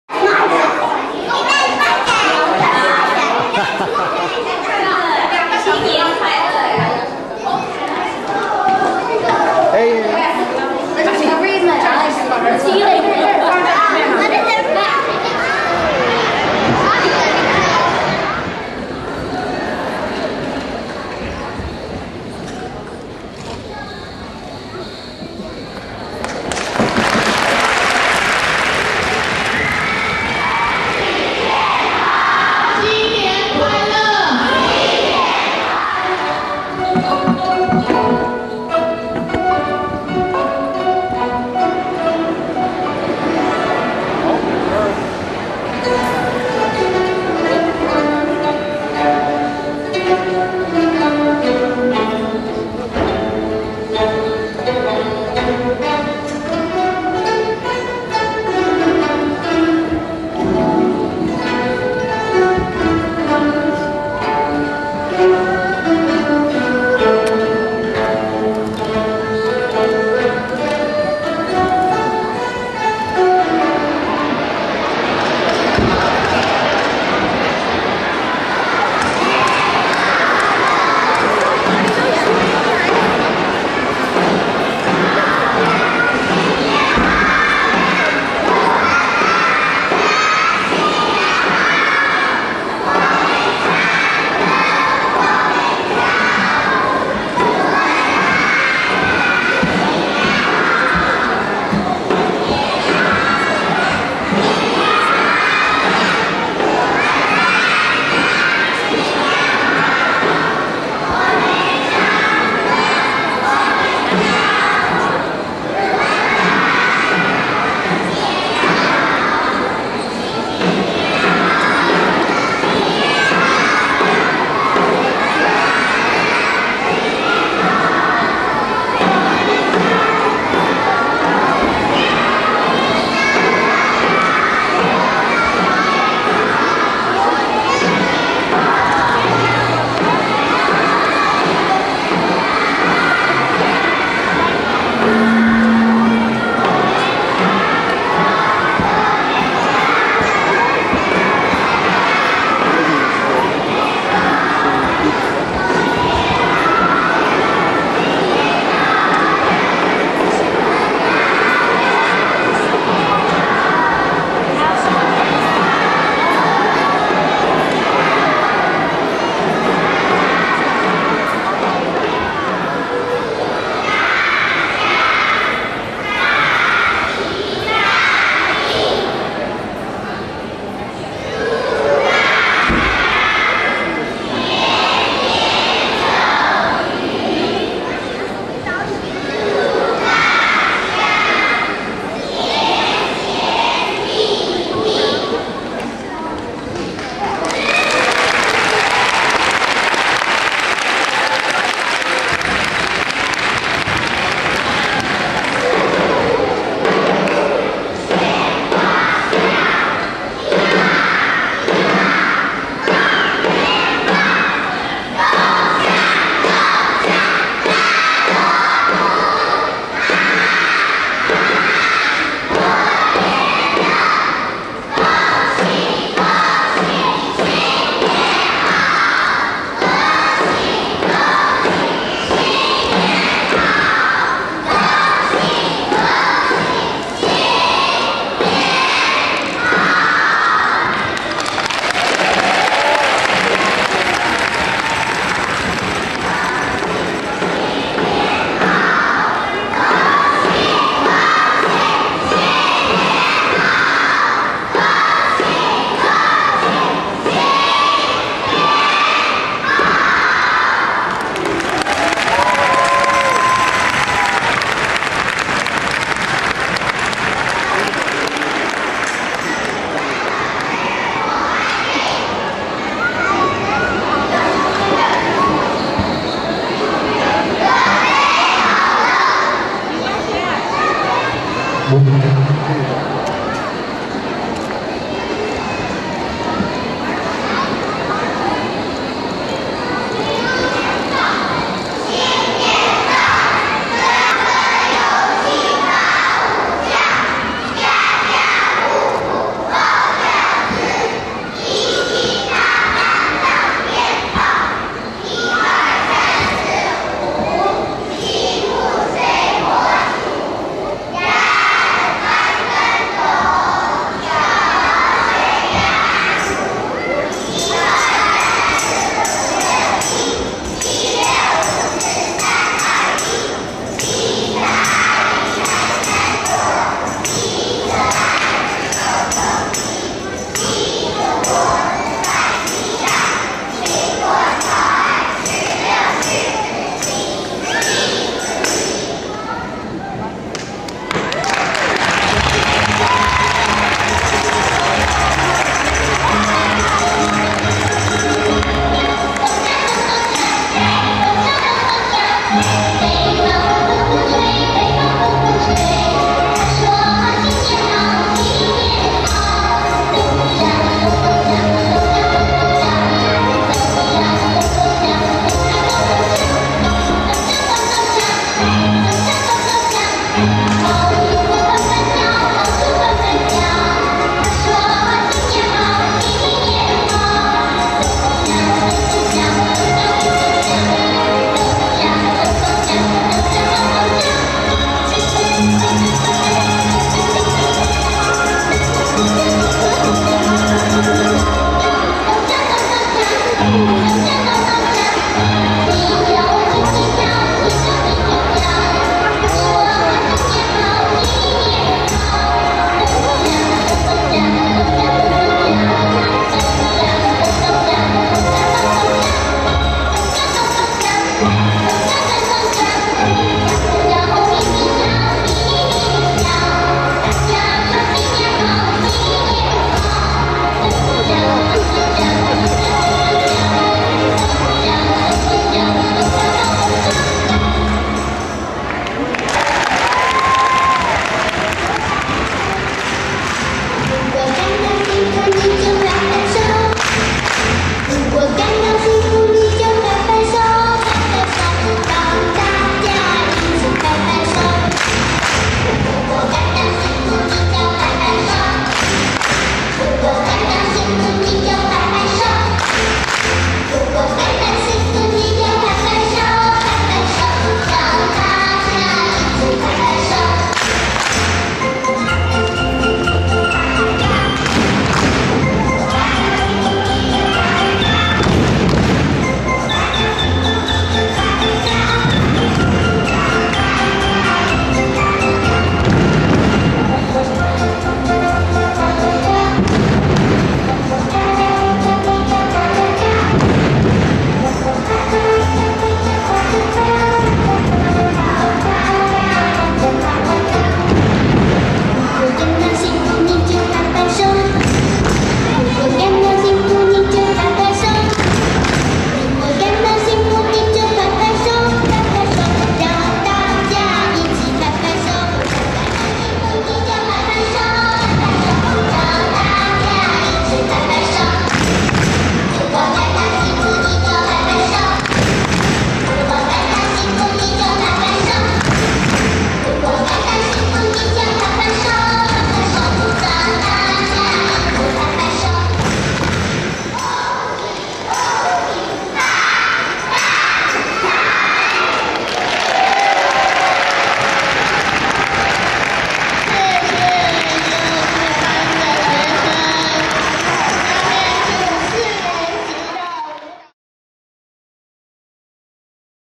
Chinese New Year 2008
CAIS celebrates the Year of the Rat (2008) new year, Julia and Helen perform